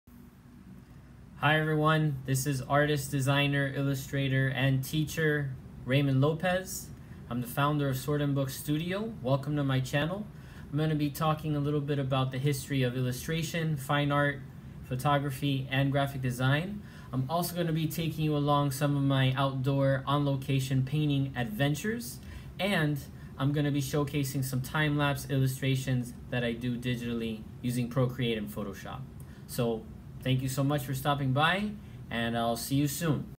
Welcome to Sword & Book Studio
Welcome to Sword & Book Studio, founded by artist and teacher Ramon Oscar Lopez.  Join me as I talk about art, illustration, graphic design, and photography.  A fine art, illustration and design studio based out of Miami, Florida.  Magnet teacher at South Miami Senior High School.